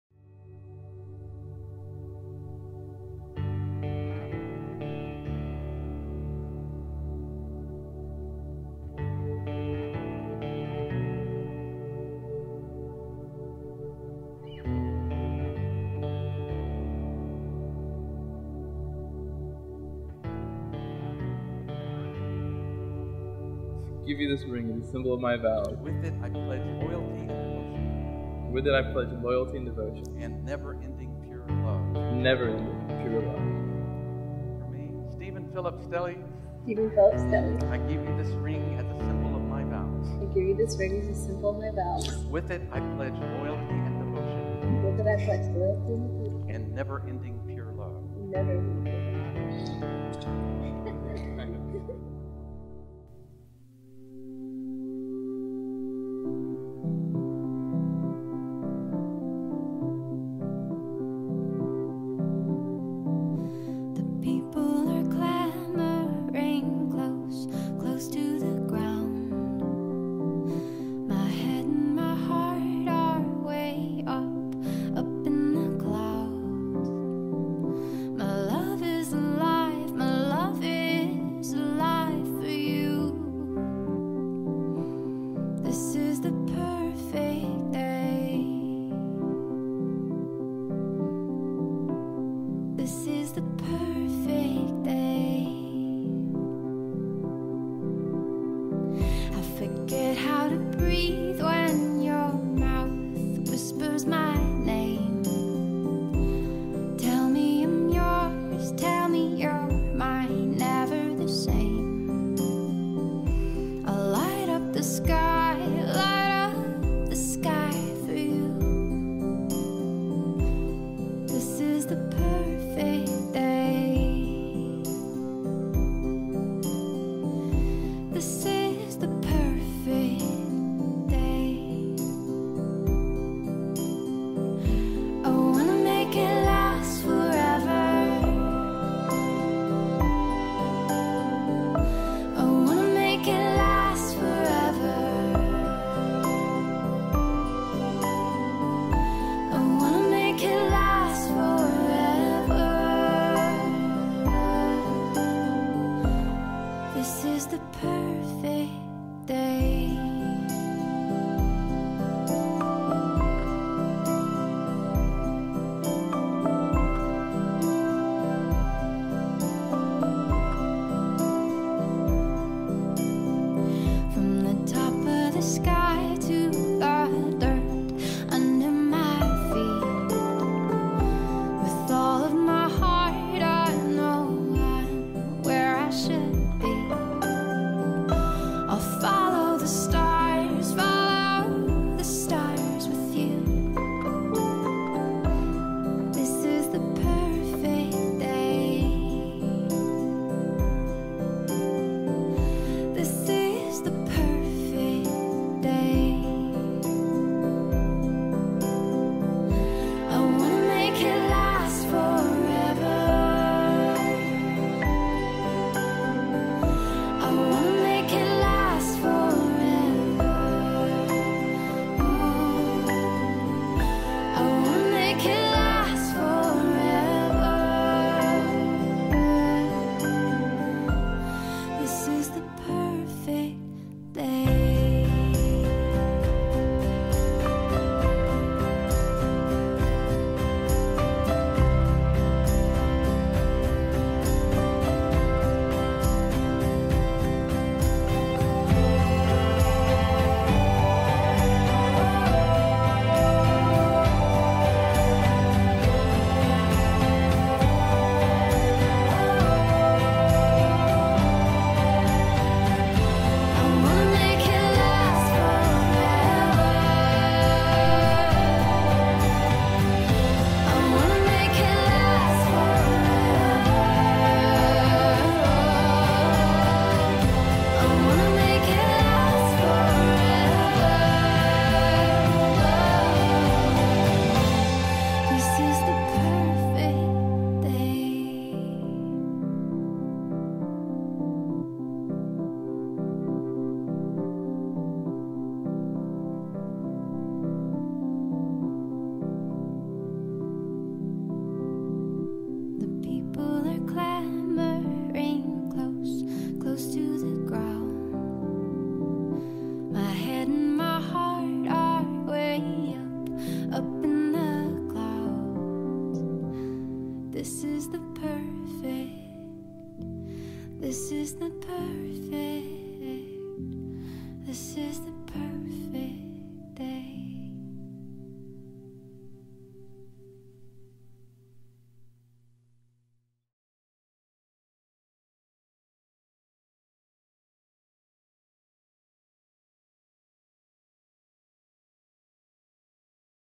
THE SPRINGS | Stelly Wedding | Magnolia, Texas
The Stelly wedding at THE SPRINGS in Magnolia was nothing shy of amazing. With the beautiful scenery and venue, its sure to amaze. We can provide you with the best amenities and details to have your dream wedding.

The Mays/Stelly wedding highlight from December 29th, 2013.